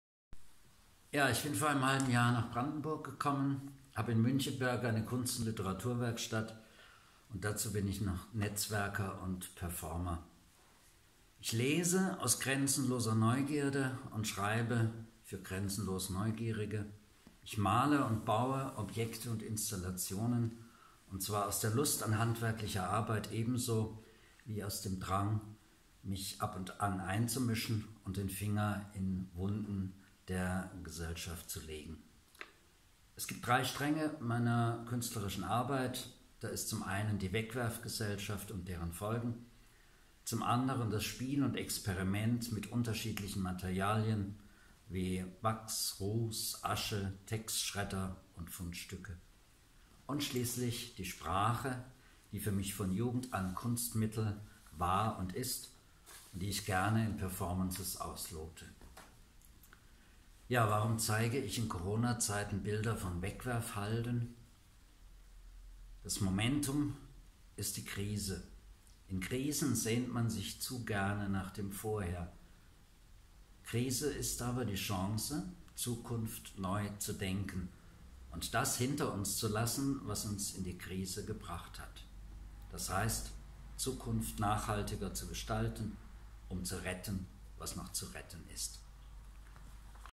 Volkmar Köhler
Bin vor einem halben Jahr nach Brandenburg gekommen und habe in Müncheberg eine Kunst&Literaturwerkstatt, bin dazu analoger Netzwerker und Performer.
Ich lese aus grenzenloser Neugierde und schreibe für andere grenzenlos Neugierige.
Ich male und baue Objekte und Installationen aus der Lust an handwerklicher Arbeit ebenso wie dem Drang, mich ab und an einzumischen und den Finger in gesellschaftlixhe Wunden zu legen.
Es gibt drei Hauptstränge meiner künstlerischen Arbeit. Das ist zum einen die Wegwerfgesellschaft und deren Folgen, zum Anderen das Spiel und Experiment mit unterschiedlichen Materialien wie z.B. Wachs, Ruß, Asche, Textschredder und Fundstücken aller Art.
Und schließlich die Sprache, die für mich von Jugend an Kunstmittel war und ist und die ich gerne in Performancetexten auslote.
Warum zeige ich in Corona-Zeiten Bilder von Wegwerfhalden?
Das Momentum ist Krise. In Krisen sehnt man sich zu gerne nach dem Vorher.
Krise ist aber die Chance, Zukunft neu zu definieren und das hinter uns zu lassen, was uns in die Krise gebracht hat, d.h. Zukunft nachhaltiger zu gestalten, um zu retten, was noch zu retten ist.

Przybyłem do Brandenburgii pół roku temu i prowadzę warsztaty artystyczno-literackie w Münchebergu, dyiaajc jednocyenie jako performer i osoba nawiązująca kontakty analogowo.
Czytam z bezgranicznej ciekawości i piszę dla innych bezgranicznie zaciekawionych ludzi.
Maluję i buduję obiekty i instalacje z pragnienia kunsztu, jak również z pojawiającej się czasem chęci ingerowania w świat i wkładania palca w skomplikowane społecznie rany.
Istnieją trzy główne wątki mojej pracy artystycznej. Jednym z nich jest społeczeństwo wyrzucające wraz z konsekwencjami takiego marnotrawstwa, drugim jest gra i eksperymentowanie z różnymi materiałami, takimi jak wosk, sadza, popiół, papier przepuszczony przez niszczarkę oraz wszelkiego rodzaju przedmioty znalezione.
I wreszcie język, który był i jest dla mnie środkiem sztuki od czasów młodości i który lubię zgłębiać w tekstach performance.
Dlaczego pokazuję zdjęcia jednorazowych wysypisk w czasach Corony?
Impulsem jest tu kryzys. W kryzysach za bardzo tęskni się za tym, co było wcześniej.
Kryzys jest jednak szansą na przedefiniowanie przyszłości i pozostawienie za sobą tego, co nas do niego doprowadziło, tzn. uczynienie przyszłości bardziej zrównoważoną, aby uratować to, co jeszcze można uratować.